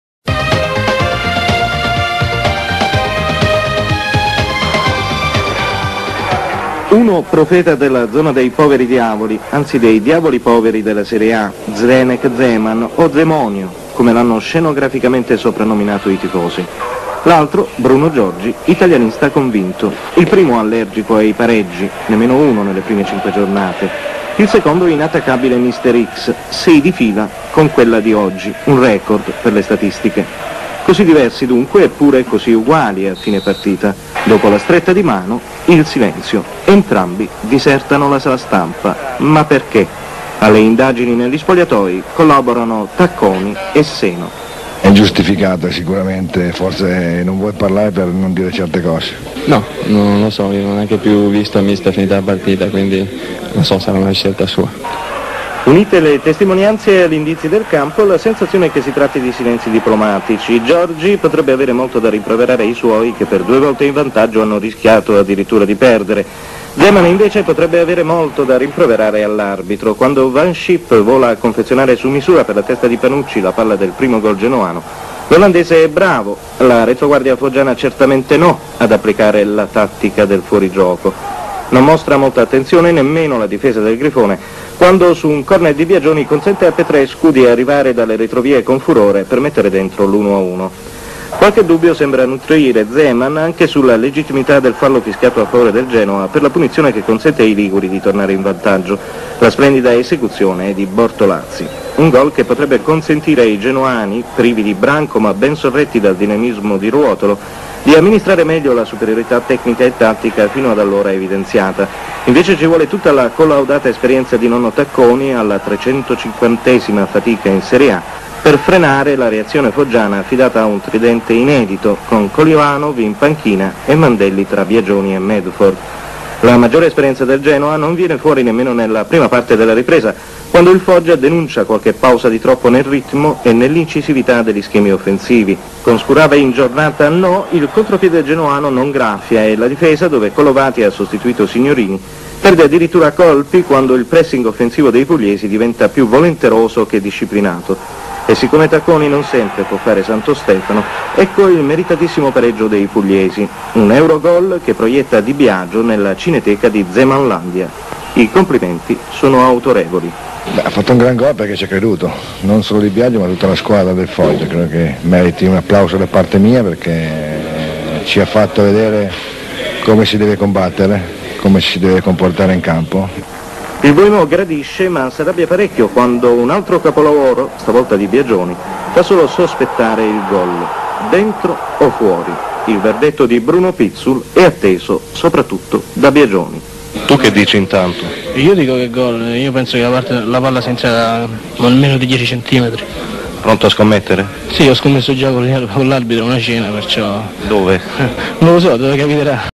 Serie A 1992-1993, day 06 Foggia - Genoa 2-2 (Panucci, Petrescu, Bortolazzi, Di Biagio)
Petrescu 26, Di Biagio 77 - Panucci 17, Bortolazzi 31